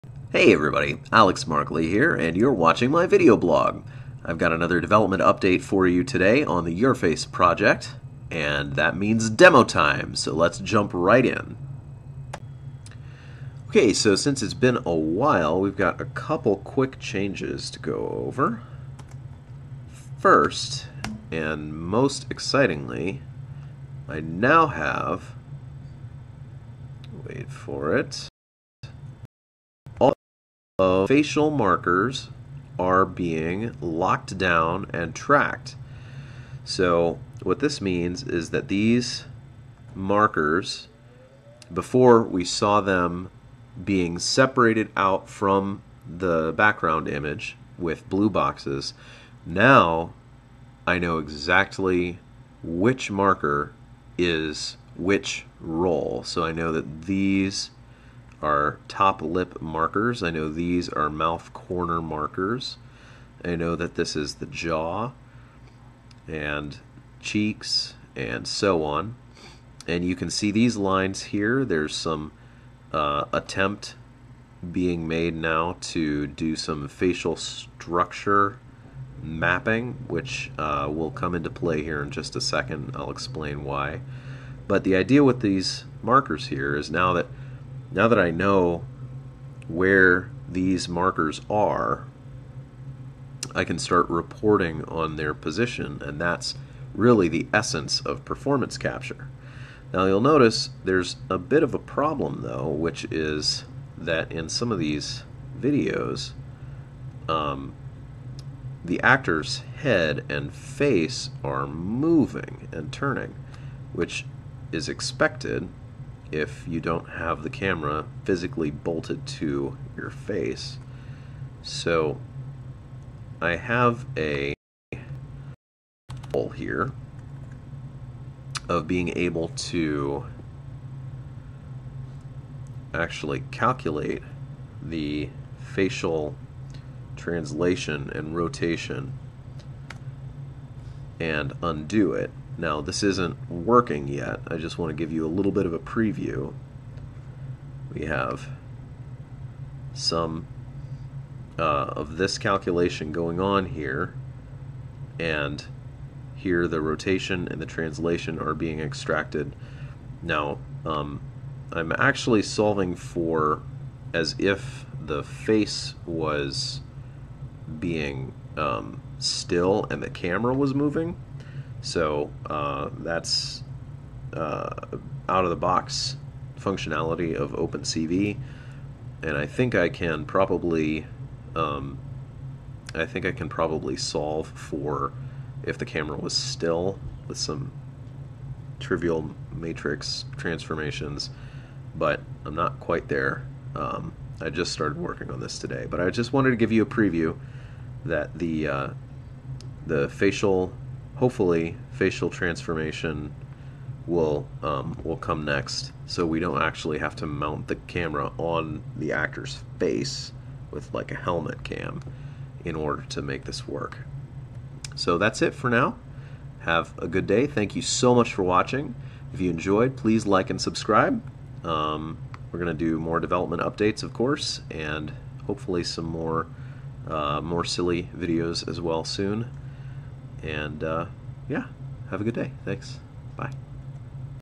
YerFace! - Marker Identification
Managed to map out and identify each marker on the face. Next up? Facial translation and rotation!

Remember, YerFace is an Open Source project hosted on Github. Comments and collaboration are welcome!